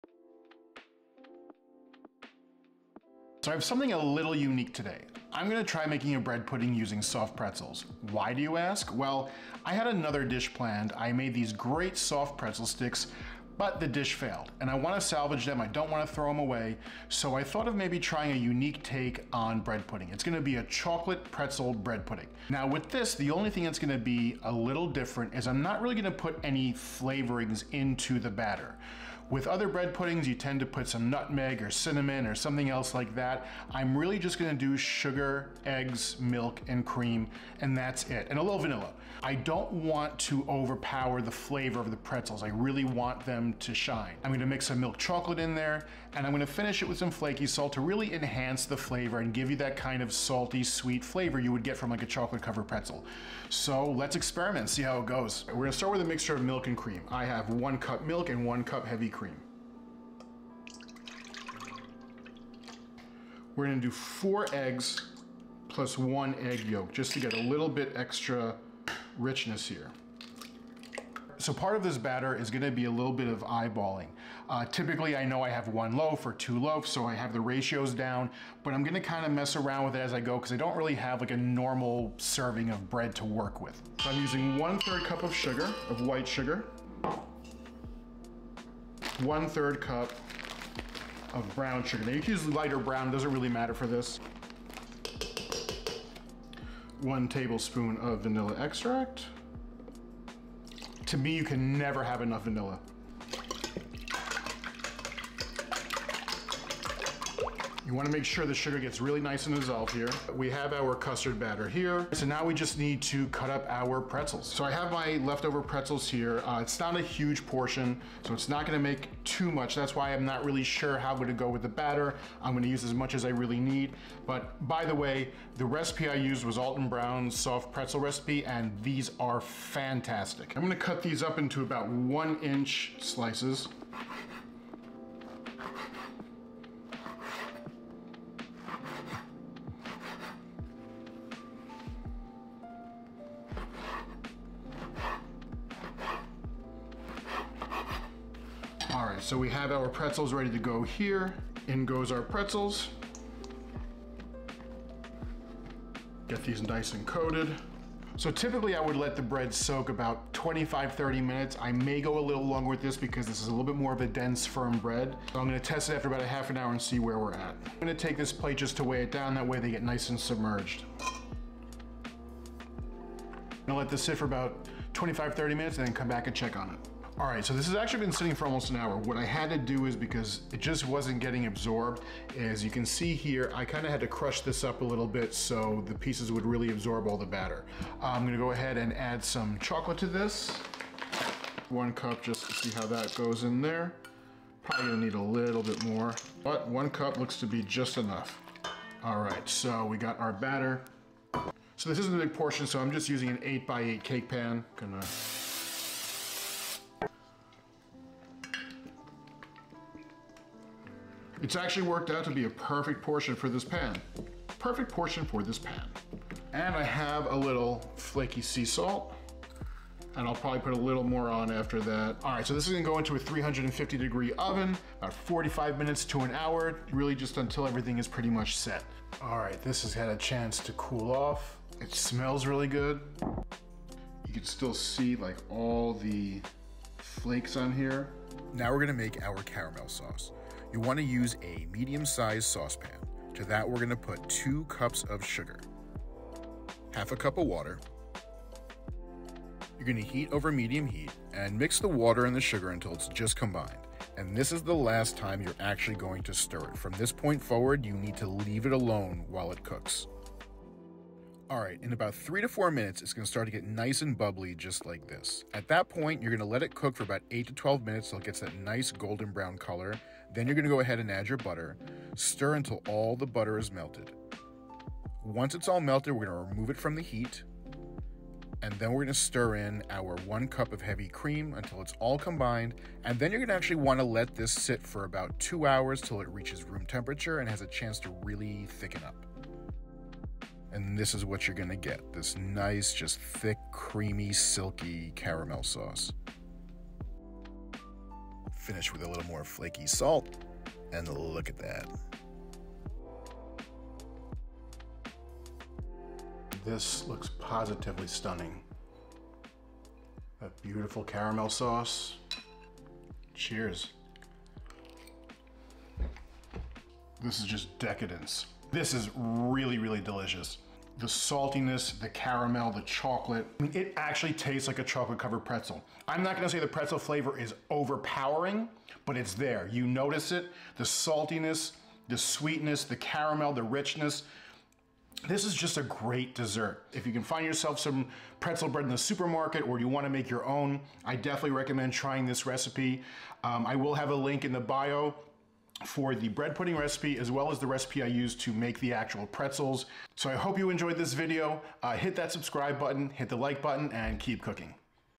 Chocolate Pretzel Bread Pudding with Caramel Sauce
This bread pudding uses soft pretzels and chocolate. It's very reminiscent of a chocolate covered pretzel.

This recipe is slightly different than the one in my video since the video used partial ingredients.

You can use store-bought soft pretzels or pretzel bread, or you can make your own pretzels. The recipe I used was Alton Brown's soft pretzel recipe:

Bread Pudding Recipe:
Soft Pretzels (about 20 oz. - the equivalent of a loaf of bread)
1.5 Cups Milk
1 Cup Heavy Cream
1/2 Cup Sugar
1/3 Cup Brown Sugar (light or dark)
4 Eggs
2 Egg Yolks
1 Tbsp Vanilla Extract
1.5 Cups Milk Chocolate (chips, disks or chopped bars) (You can use dark if you prefer, but I feel it may be too strong of a flavor in this dish)
Maldon Salt (Flakey salt)
Caramel Sauce:
1 Cup Sugar
1/4 Cup Water
6 Tbsp Butter - cut up
1/2 Cup Heavy Cream
Pinch of kosher salt
1 tsp Vanilla (Optional)
Step 1:
Prepare caramel sauce since it needs time to cool and thicken. In a medium sauce pan, over medium heat, combine sugar and water. Stir together and then leave it alone. You should not stir past this point.  The first three to five minutes it will start to bubble. Once it comes to a bubble, pay attention. You're looking for that classic dark golden brown caramel color. This should take around eight to ten minutes.

Once you've reached the desired color, add butter and whisk or stir until butter is fully melted. The caramel will bubble up a lot when you first add the butter, but keep whisking. Remove from heat, add milk and combine until smooth. Pour into a dish and allow to cool to room temperature and thickened to desired consistency. If you'd like a thicker sauce you can allow more time to cool in the fridge.

Step 2:
In a large mixing bowl, add all ingredients except for pretzels, salt and chocolate. Whisk until sugar is dissolved. Cut your pretzels in roughly one-inch chunks. Place your pretzels in the custard batter, break pieces apart with your hands. You don't want to break into very little pieces, just enough so the batter can absorb. Cover and let sit for thirty-five minutes. Preheat oven to 350 while this rests.

Step 3:
Spray a 9x13 baking dish with non-stick spray. In the mixing bowl, add the chocolate and mix. Pour mixture into baking dish. Sprinkle with two teaspoons of maldon salt, or more if you desire. Bake for 45 minutes to one hour. You're looking for the pudding to be firm and set. Not wobbly.

Step 4:
Cut into desire pieces, pour caramel sauce and finish with more maldon salt.